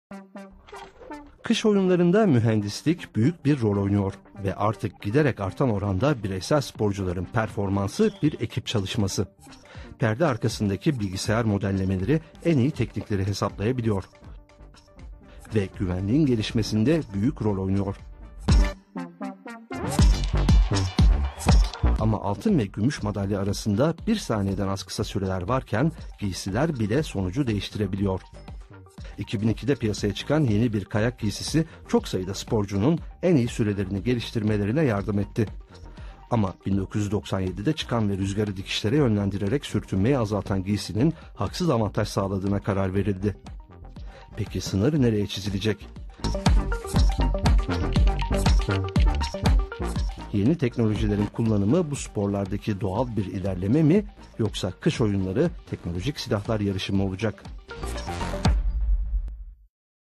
Soçi 2014: Teknoloji
Yeni teknolojilerin kullanımı spor dallarındaki doğal bir ilerleme mi yoksa, 2014 Kış Olimpiyat Oyunları teknolojik silahlar yarışı mı olacak?

datavis,olimpiyat,soçi,teknoloji